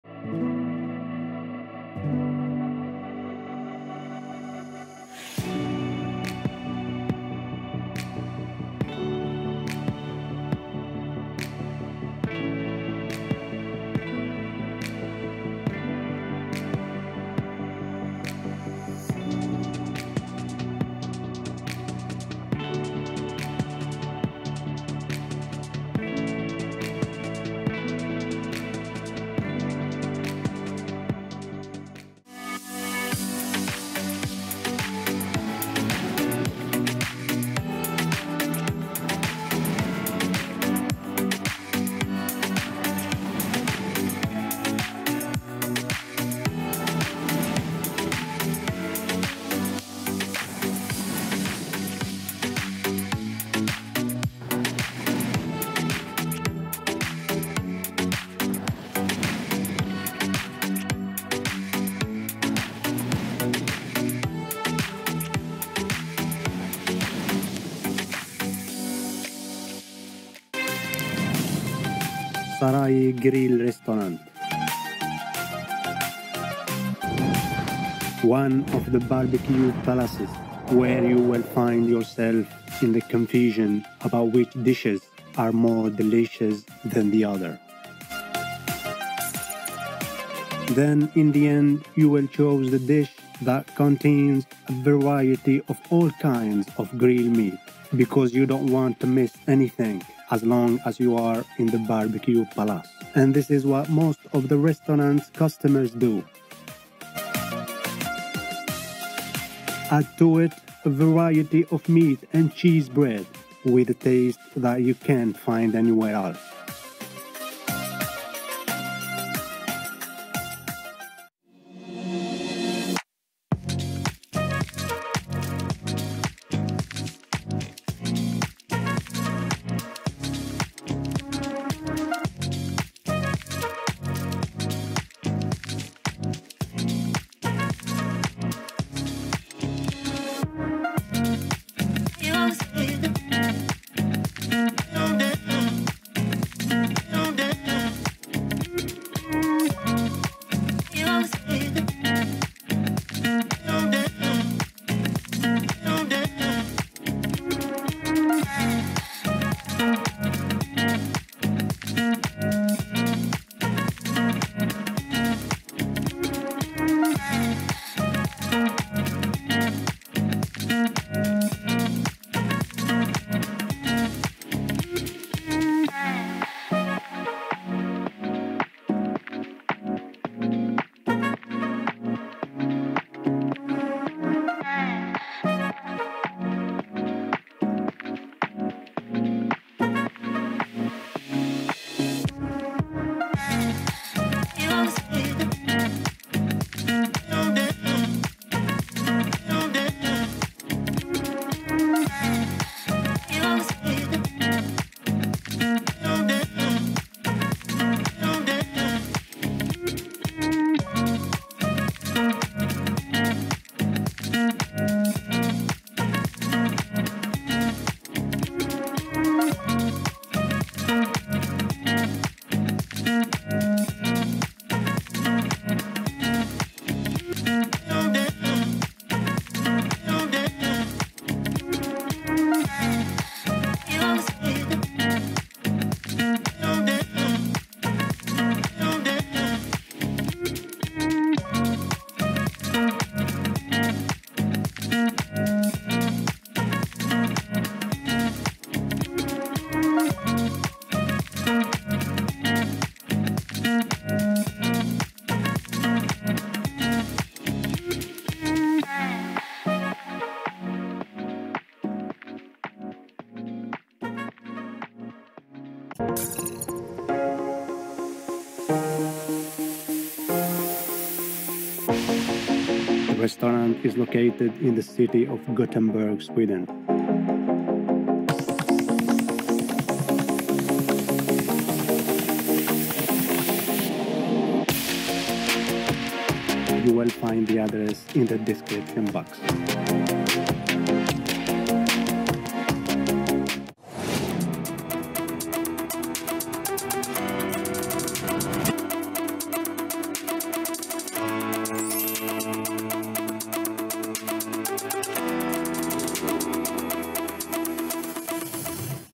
food tour sweden: SARAY Grill & kök Gothenburg
Real Turkish food is at its best test in Sweden
Saray grill & kök

Address: Alekärrsgatan 10, 415 02 Göteborg

00:00 Introduction
02:02 Menu
04:28 Address